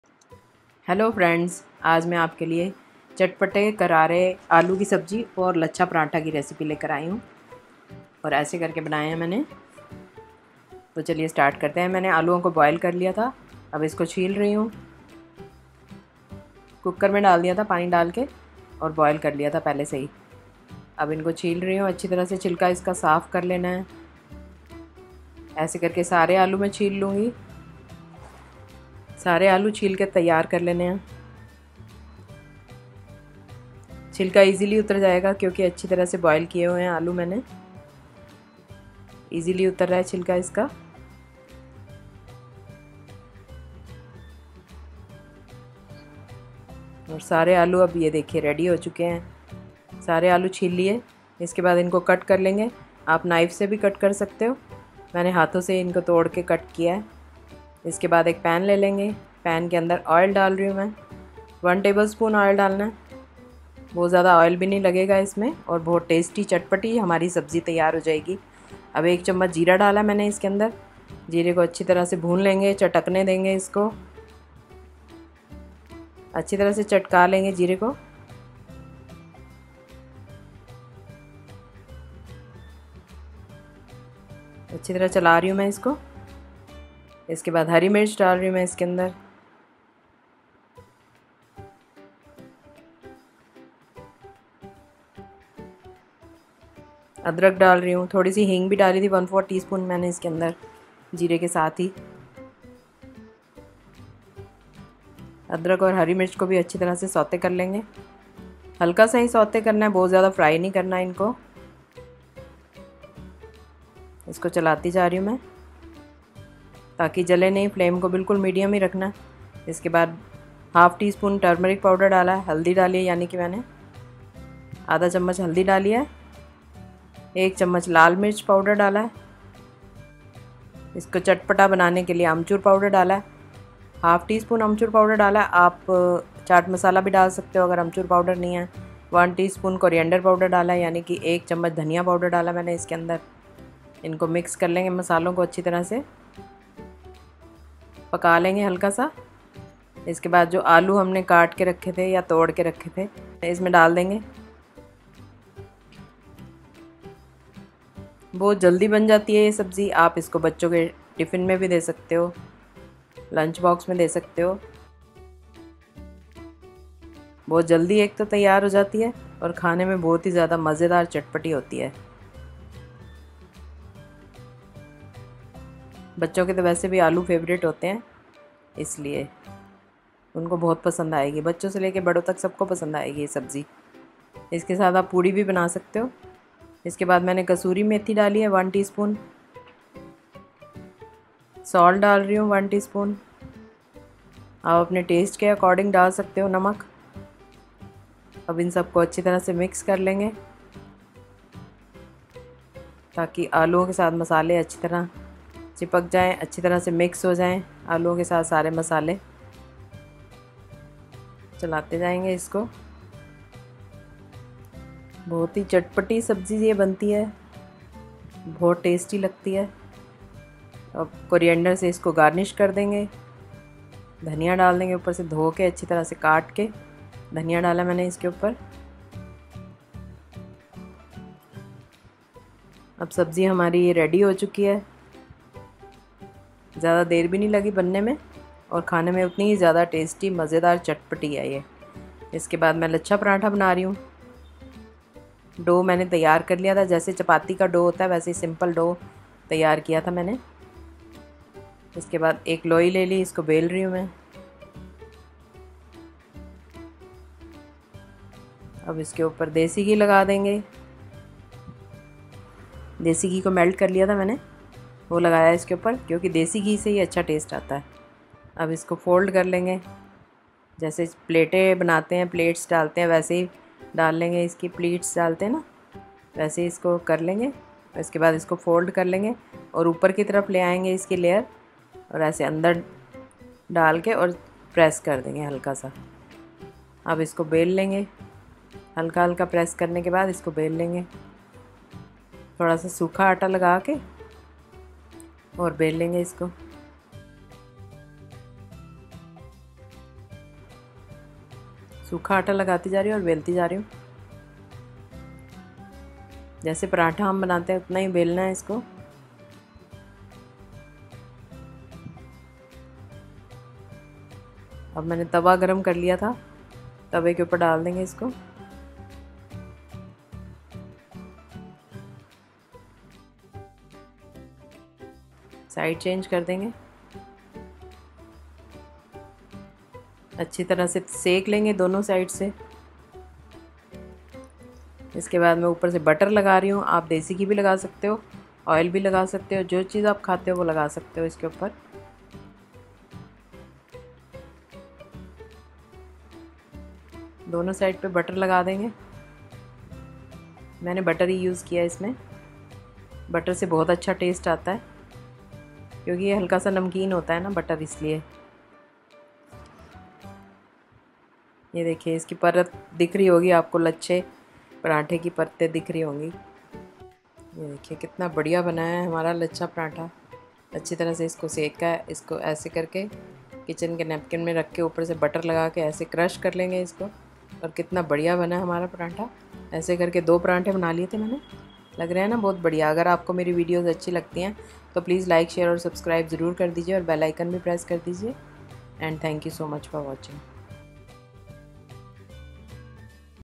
Jeera Aloo with Laccha Paratha | आलू की चटपटी सब्जी और लच्छा पराठा
🍽️𝗔𝗠𝗔𝗭𝗢𝗡 LINKS🍽️ Equipments/Products used, please check the links below for huge discounts:
Iphone 14
Fake Plants
Air Fryer

6  potato
salt to taste
1/2 tsp turmeric powder
1 tsp chilli flakes
1/2 tsp dry mango powder
1/2 tsp kasoori methi
1 tbsp oil
1 tbsp desi ghee
1 tsp chop ginger
1 tsp chop green chilli
1 tsp cumin seeds
1 tsp coriander powder
1 tsp chopped Coriander
1 pinch asafoetida

For suggestions please feel free to "COMMENT" and also "SHARE" the video to show your support.